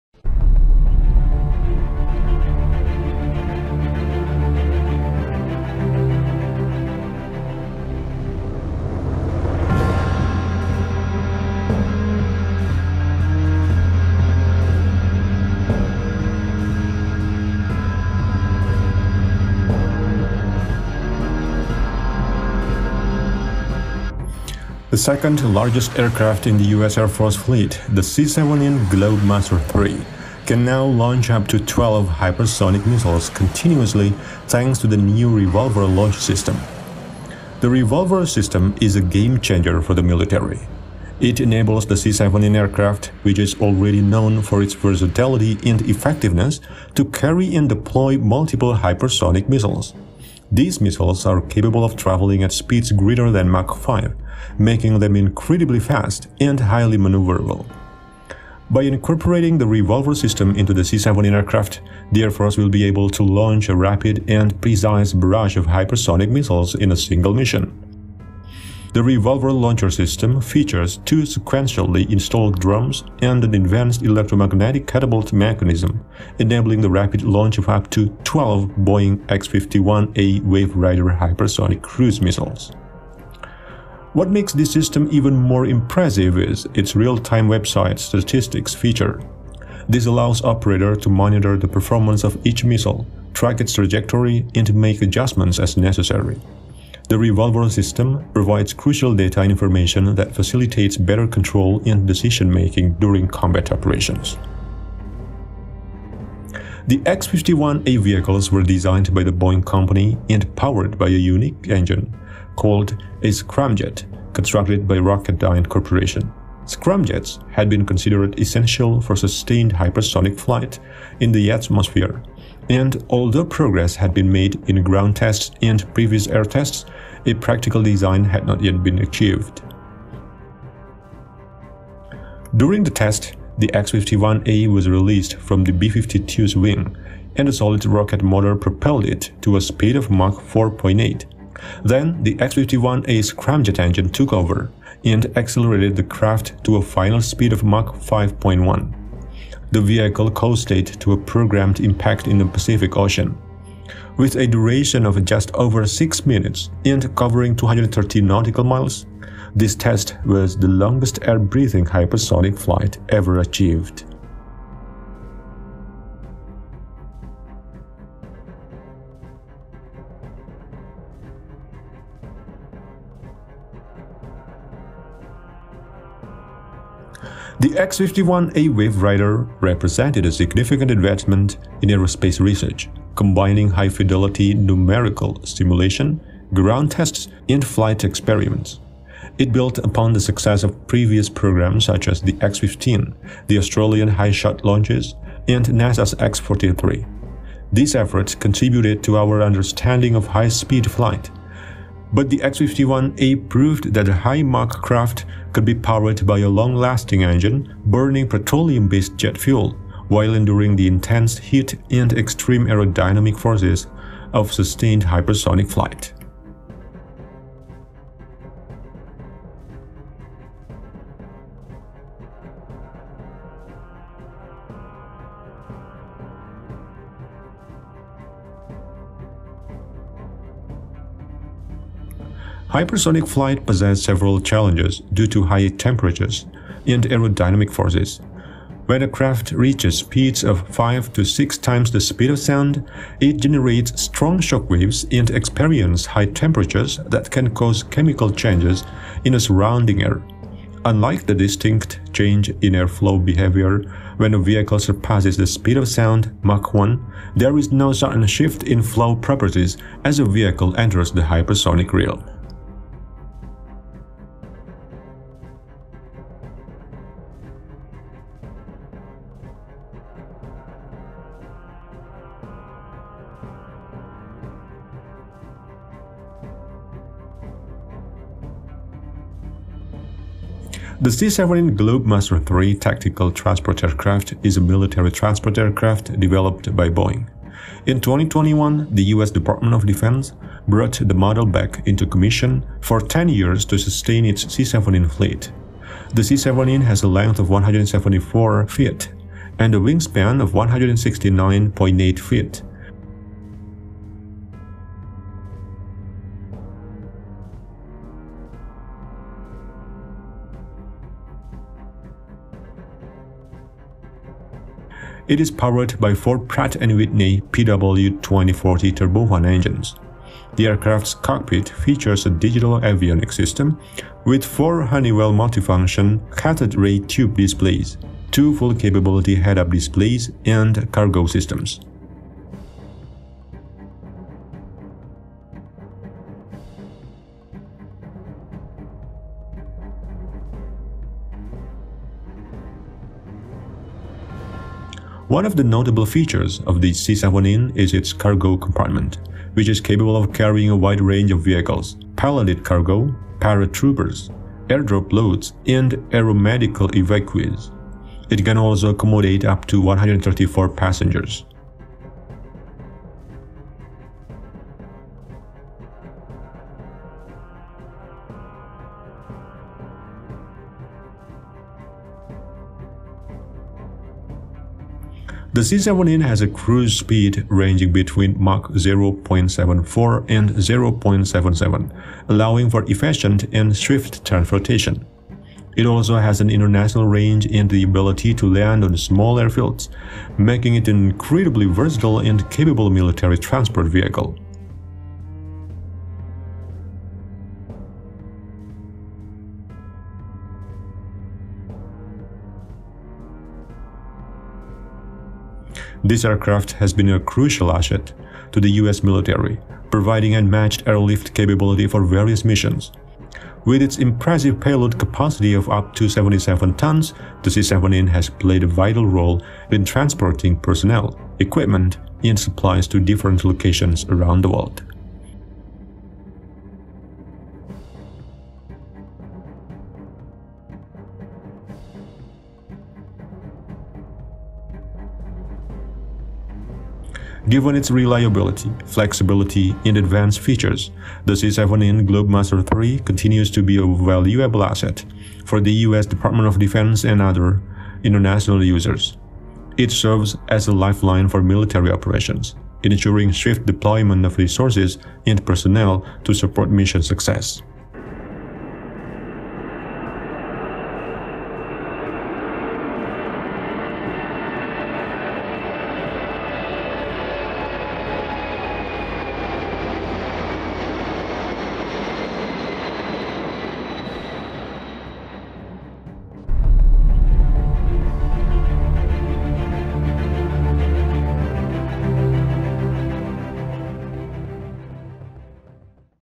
Super's idea of ​​incorporating the REVOLVER system into the C-17 plane to launch hypersonic-missile
The second largest aircraft in the US Air Force fleet, the C-17 Globemaster III, can now launch up to 12 hypersonic missiles continuously, thanks to the new REVOLVER Launch System.

The REVOLVER system is a game-changer for the military, features two sequentially installed drums and an advanced electromagnetic catapult mechanism.